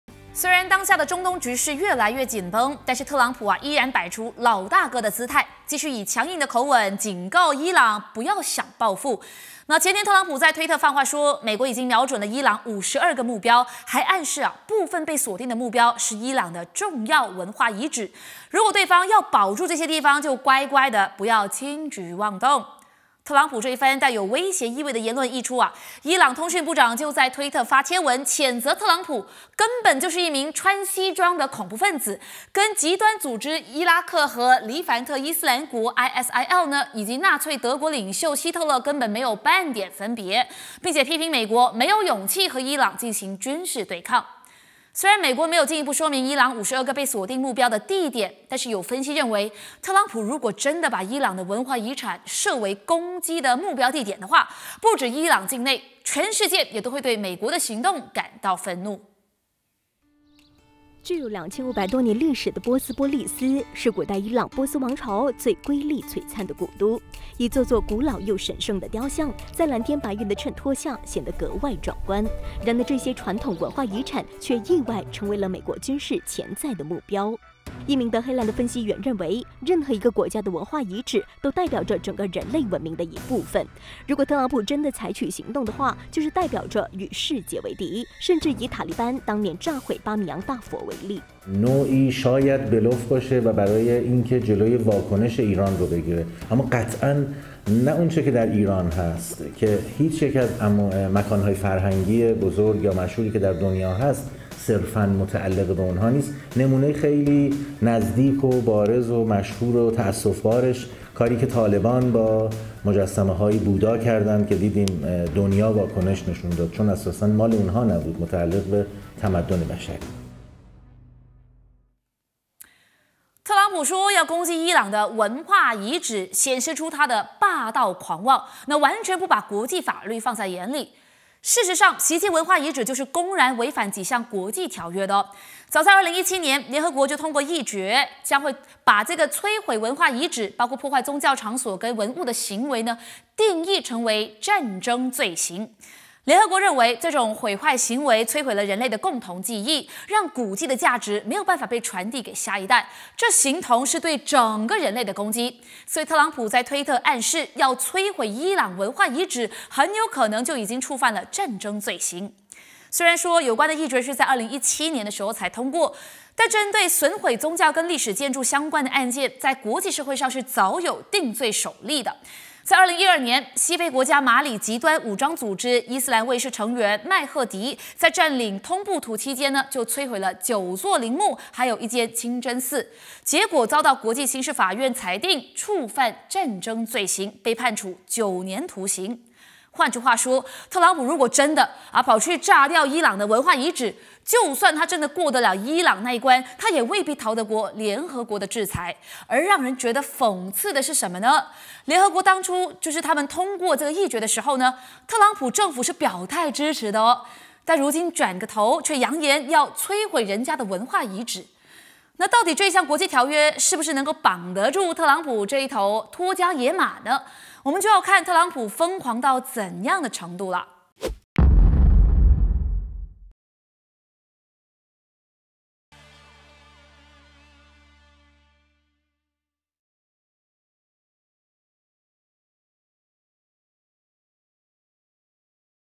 八点最热报 06/01/2020 袭击伊朗文化遗产即犯禁 特朗普公然违反国际公约?
美国总统特朗普，放话瞄准伊朗52个目标，还暗示部分属伊朗重要文化遗址，要对方不要轻举妄动报复美国。然而，早在2017年，联合国通过决议，将摧毁文化遗址定义为战争罪行，特朗普若执意摧毁伊朗文化遗址的话，就是公然违反国际条约。